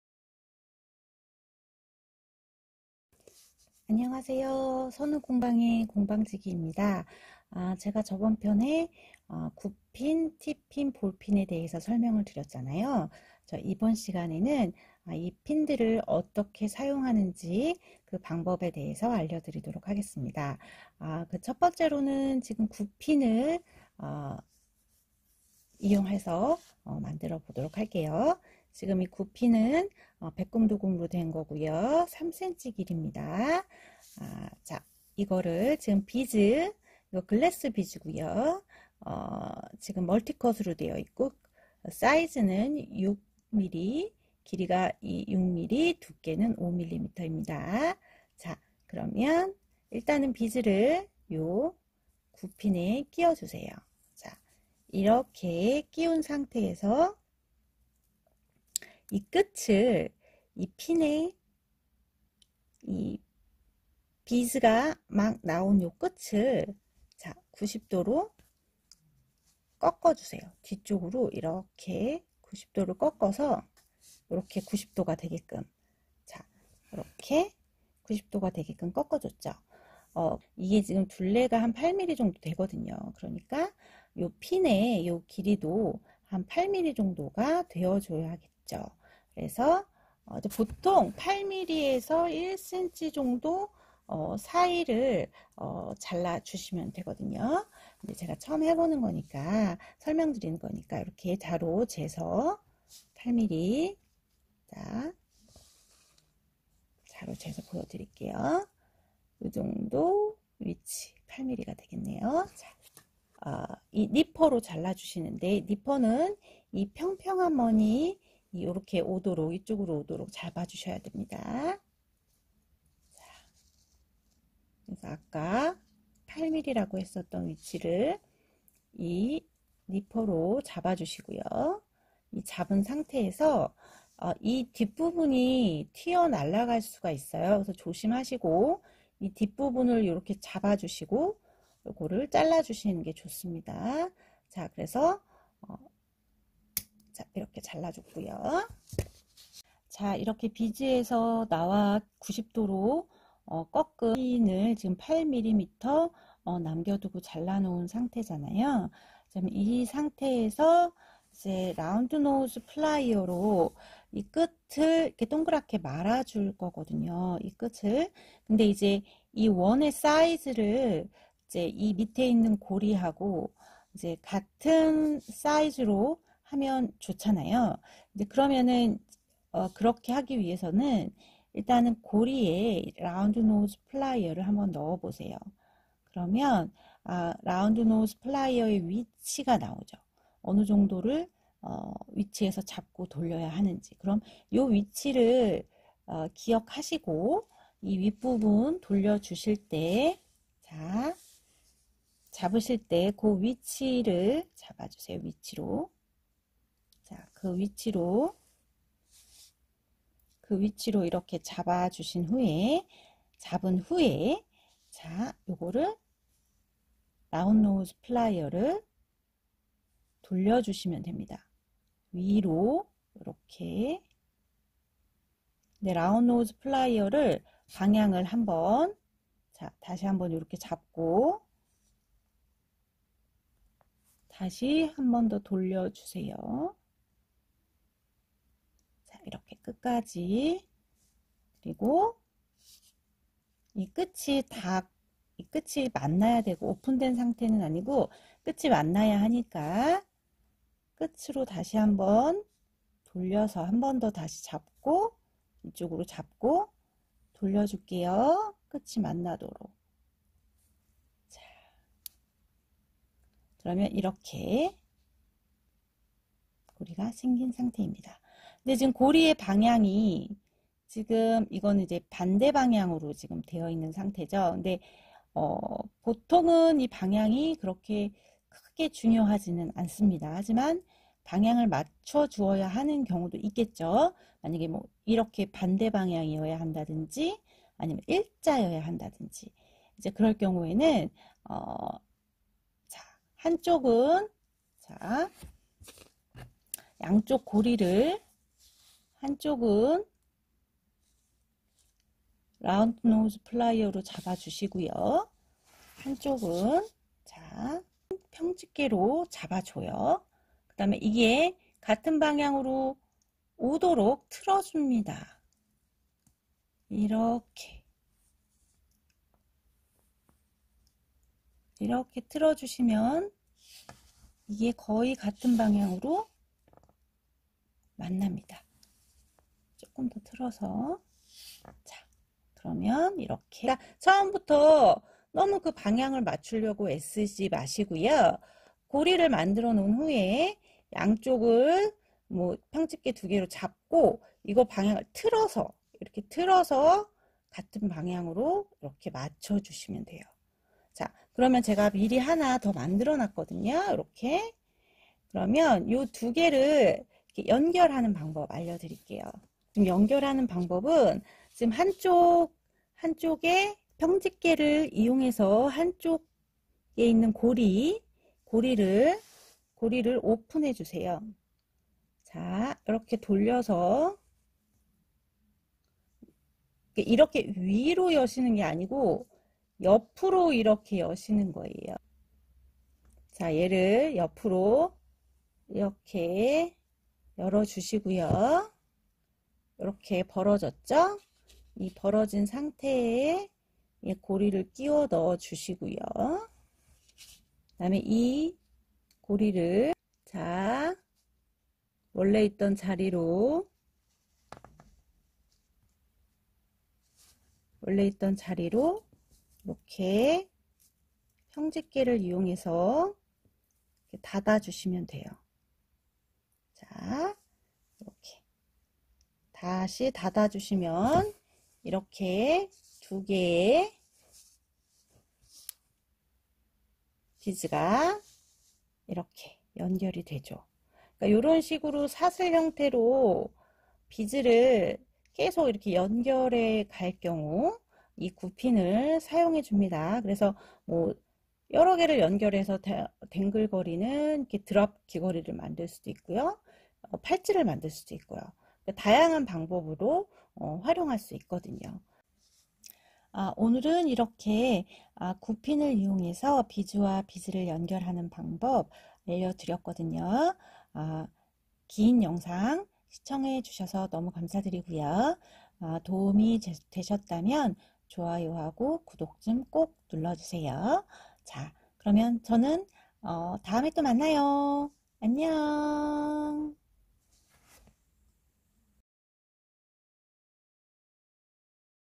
9핀 사용법
안녕하세요. 서누공방의 공방지기입니다.

오늘은 9핀을 사용하여 비즈와 비즈를 연결하는 방법을 설명해 드리겠습니다.

* 9핀
- 9핀의 사이즈는 두께 0.6mm, 길이는 3cm 입니다.
- 9핀은 아이핀(Eyepin)이 정식 명칭이며, 숫자 9 모양을 닮았다고 하여 9핀이라는 애칭으로 불리고 있습니다.
- 비즈류를 사슬처럼 연결하고 싶을 때 유용하게 쓰이는 제품입니다.
- 한번 구부린 9핀은 다시 펴줄 경우 부러질 수 있으니 사용하실 때 주의하셔야 합니다.
- 9핀 고리의 둘레가 거의 8mm라서 반대편도 비즈 밖으로 핀을 8mm 길이로 잘라줍니다.

* 글래스 비즈 멀티컷
- 유리를 가공하여 만든 비즈입니다.
- 사이즈는 6X5mm 이며 구멍은 중심의 좌우로 뚫려있습니다.

* 사용한 도구들
- 평집게
- 라운드노우즈 플라이어
- 니퍼

좀더 다양한 액세서리의 내용을 보고 싶으시면 저의 NAVER 블로그 '서누공방'에 방문해 주세요.